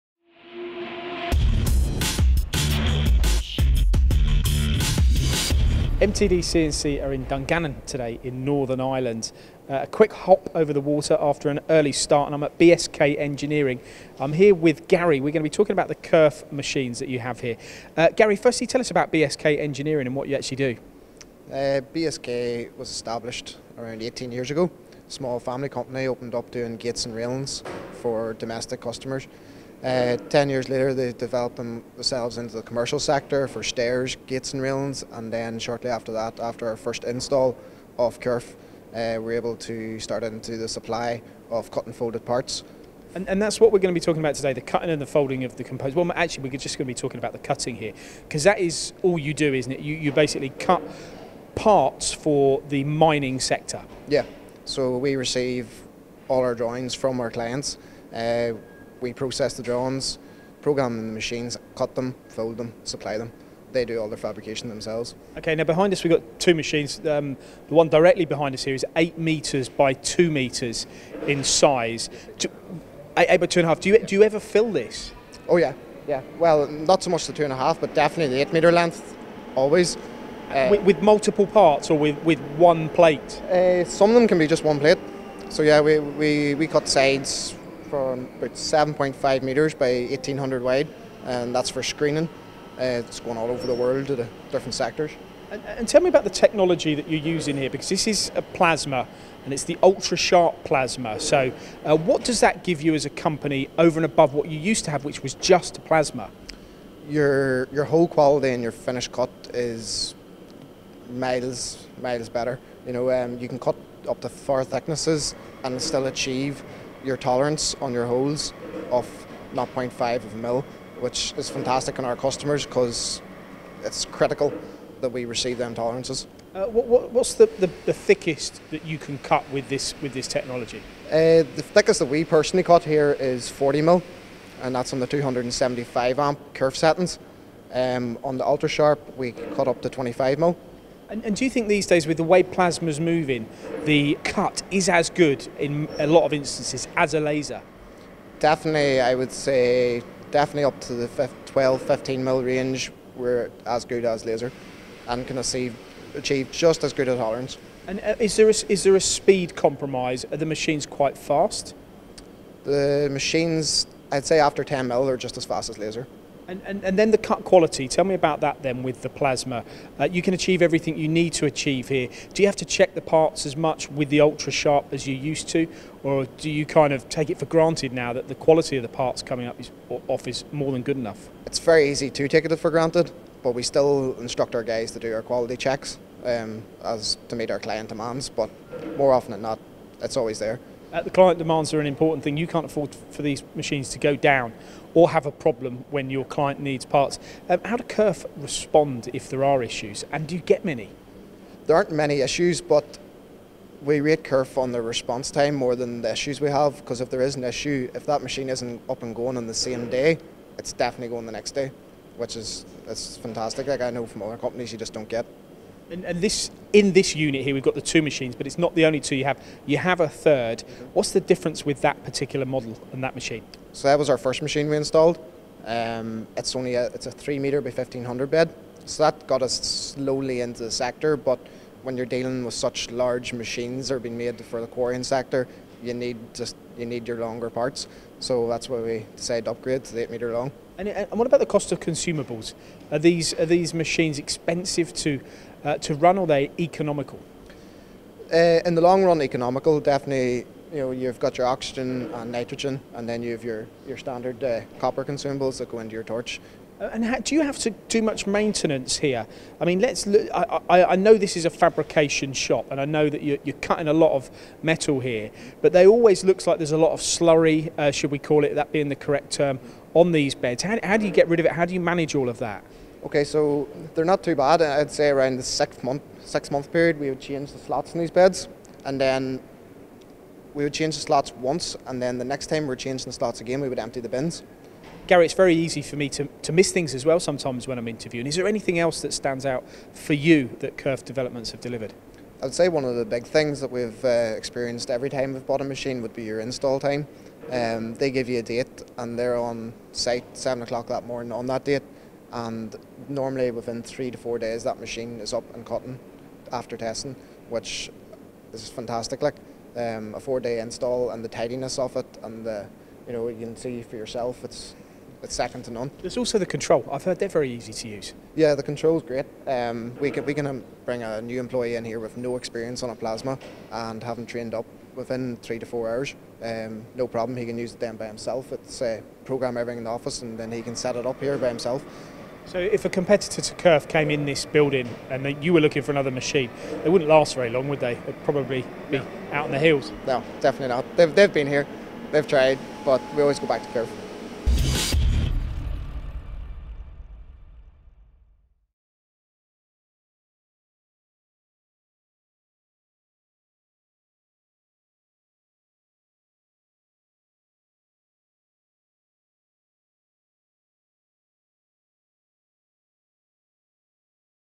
BSK Engineering in Northern Ireland Specialise in cutting quality
A recent trip to Northern Ireland saw Paul visited BSK Engineering. A company who have recently invested in another Kerf ultra sharp Plasma machine. This particular model provides much better cut quality and surface finish as Gary explains.